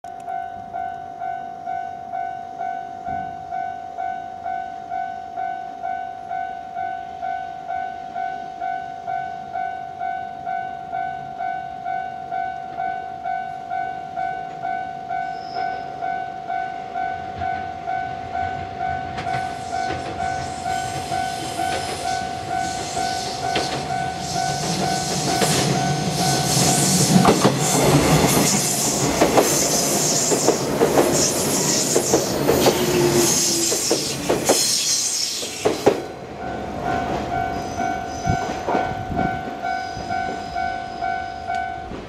折尾駅の短絡線を通過 415系 福北ゆたか線
鹿児島本線と筑豊本線を繋ぐ短絡線
622H、撮影は日曜日
将来的には折尾駅本屋高架化完了後に置き換えられ
線路ごと撤去される事となります。
尚、福北ゆたか線とは黒崎～折尾～桂川～博多間の運転系統の愛称です。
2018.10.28
Panasonic HC-V520M 撮影